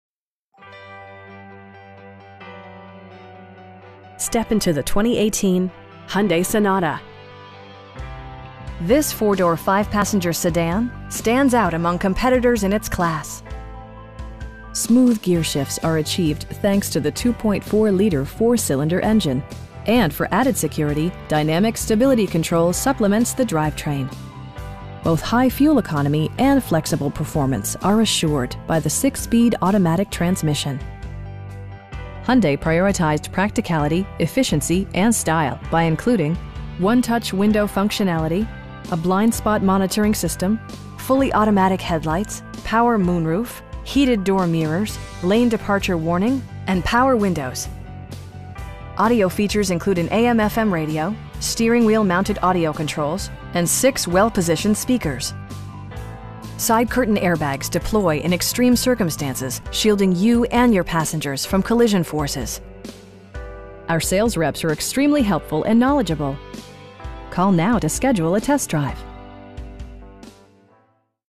2018 Hyundai Sonata Sport in Oklahoma City, OK 73139
Bob Howard Hyundai
613 S.W. 74th

Step into the 2018 Hyundai Sonata. This 4 door, 5 passenger sedan stands out among competitors in its class! Smooth gearshifts are achieved thanks to the 2.4 liter 4 cylinder engine, and for added security, dynamic Stability Control supplements the drivetrain. Both high fuel economy and flexible performance are assured by the 6 speed automatic transmission. Hyundai prioritized practicality, efficiency, and style by including: 1-touch window functionality, a blind spot monitoring system, fully automatic headlights, power moon roof, heated door mirrors, lane departure warning, and power windows. Audio features include an AM/FM radio, steering wheel mounted audio controls, and 6 well positioned speakers. Side curtain airbags deploy in extreme circumstances, shielding you and your passengers from collision forces. Our sales reps are extremely helpful & knowledgeable. They'll work with you to find the right vehicle at a price you can afford.